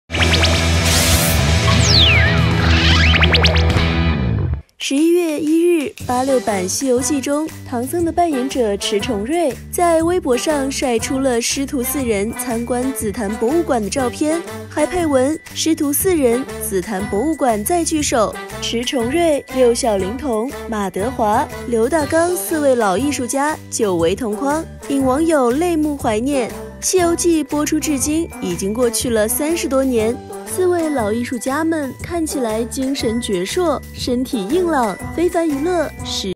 86版《西游記》師徒四人久違同框 六小齡童再現經典動作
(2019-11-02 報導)

86版《西游記》師徒四人久違同框 六小齡童再現經典動作